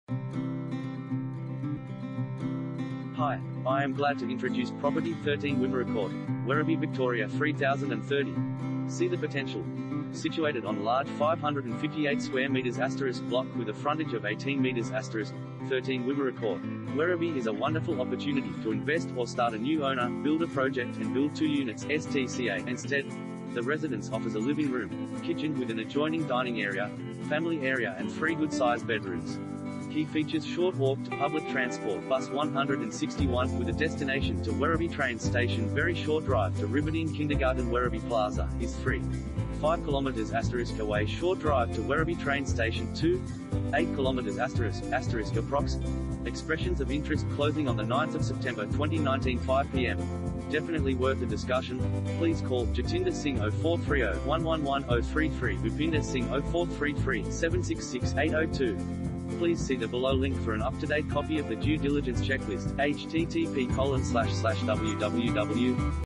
13 Wimmera Court, Werribee VIC 3030 | AuPropertyReport.Com
13 Wimmera Court, Werribee VIC 3030
Property Type: house
Bedrooms: 3
Bathrooms: 3
Car Space: 1
See the Potential!